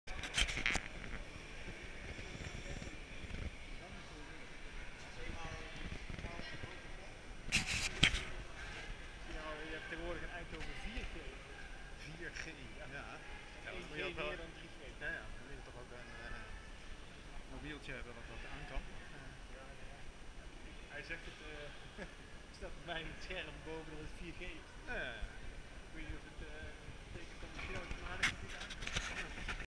Climbing in Monk
Overview of the boulderhall in Eindhoven